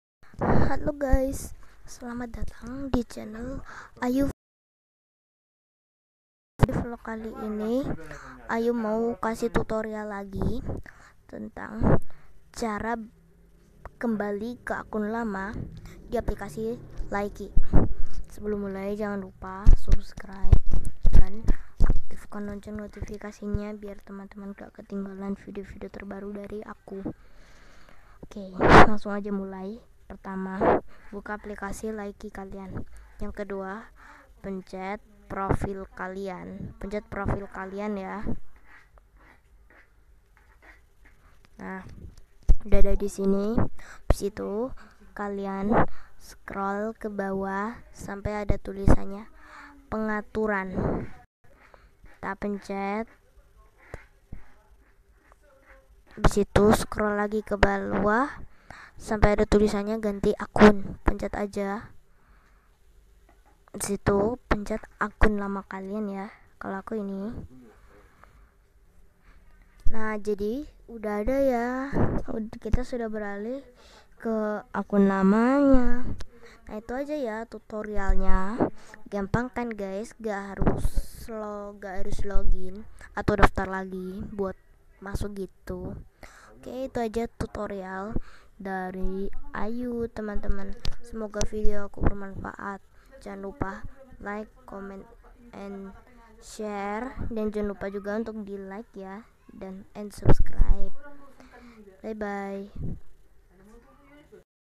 CARA KEMBALI KE AKUN LAMA DI APLIKASI LIKEE✨ By:Ayu F.H🌹
CARA KEMBALI KE AKUN LAMA DI APLIKASI LIKEE✨

tanpa daftar ulang untuk ke Akun Lama!

By:Ayu F.H🌹

smoga klian suka video aku, klo suka like y guys❤

Apk Record : Mobizen

Apk Edit Video :YouCut